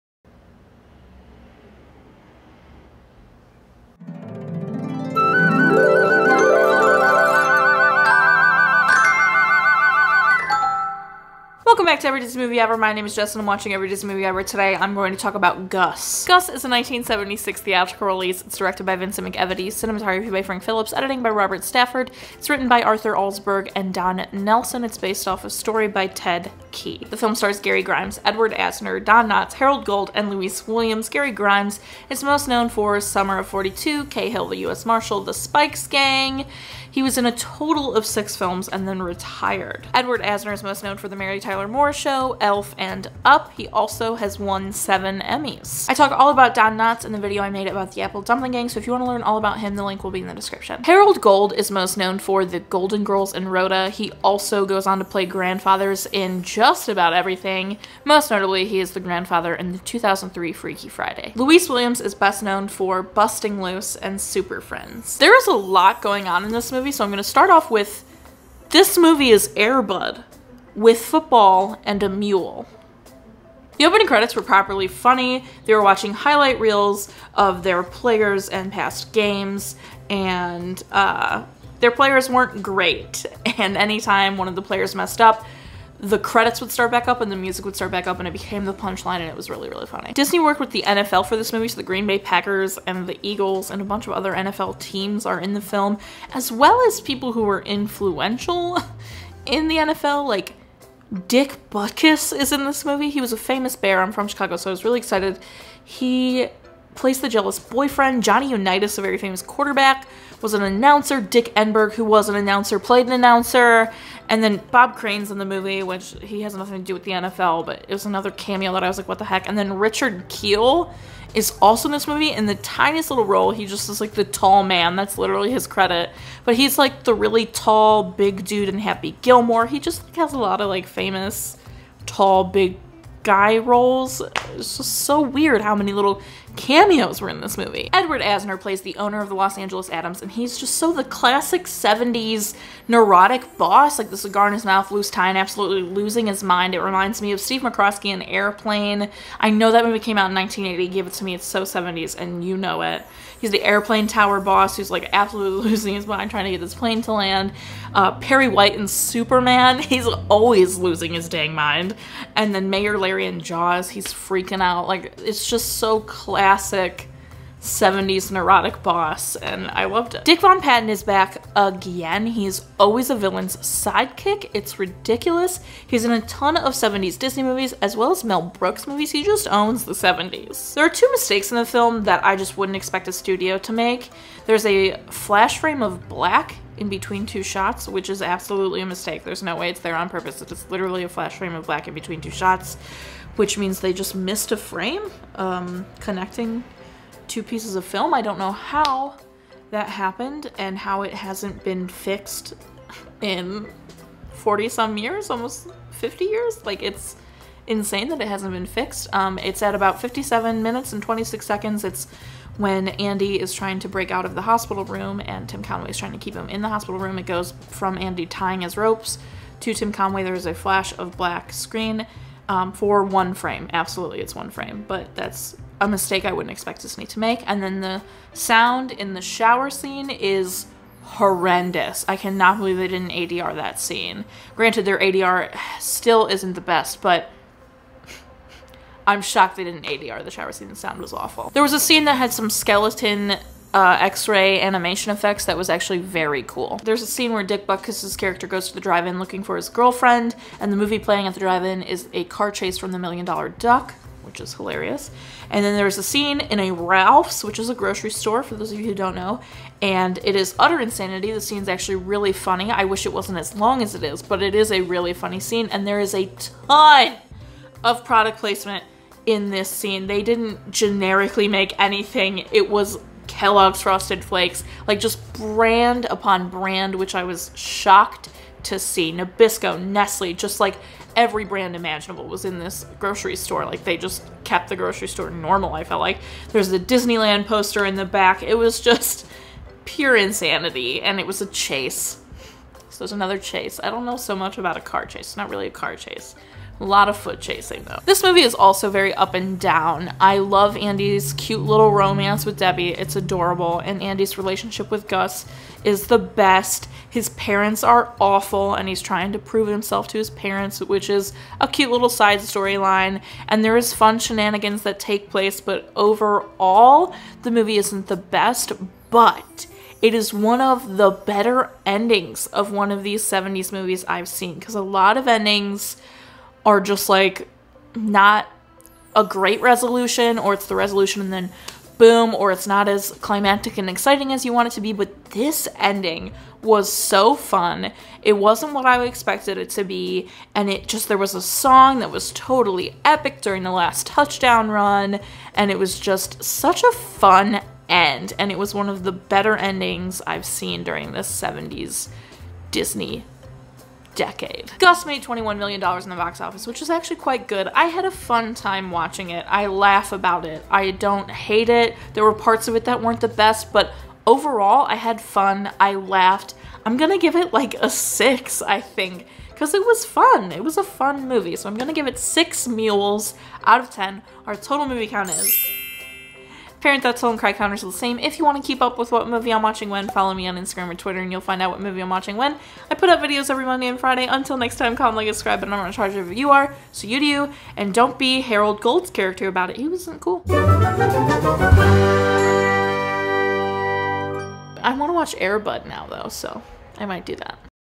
Every Disney Movie Ever: Gus
Gus is a 1976 theatrical release.

Director: Vincent McEveety
DP: Frank Phillips
Editor: Robert Stafford
Writer: Arthur Alsberg and Don Nelson
Story: Ted Key
Lead Actor: Gary Grimes, Edward Asner, Don Knotts, Harold Gould and Louise Williams.

TOTALS:
Movies watched: 166
Parent Deaths: 32
Cry Count: 8